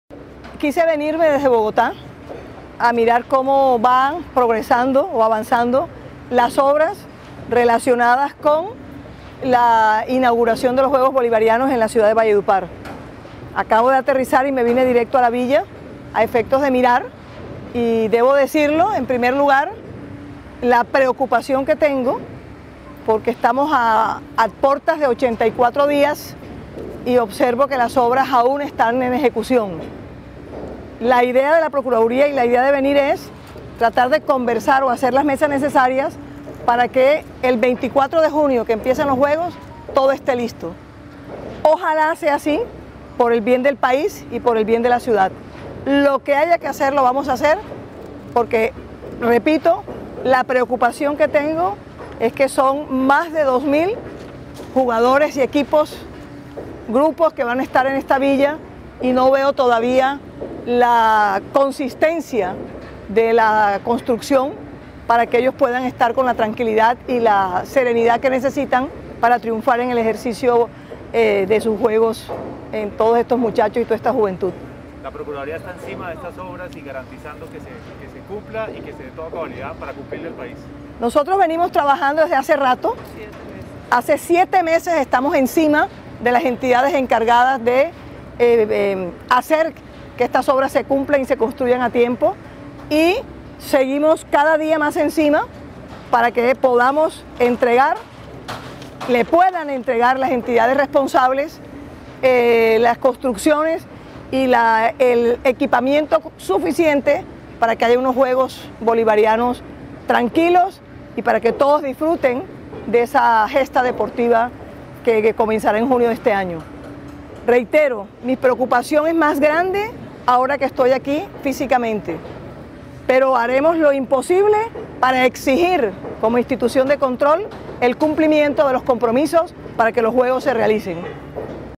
Procuradora le jaló las orejas a Valledupar por el atraso de las obras de los Juegos Bolivarianos
Declaraciones en la Villa Bolivariana, 31 Marzo 2021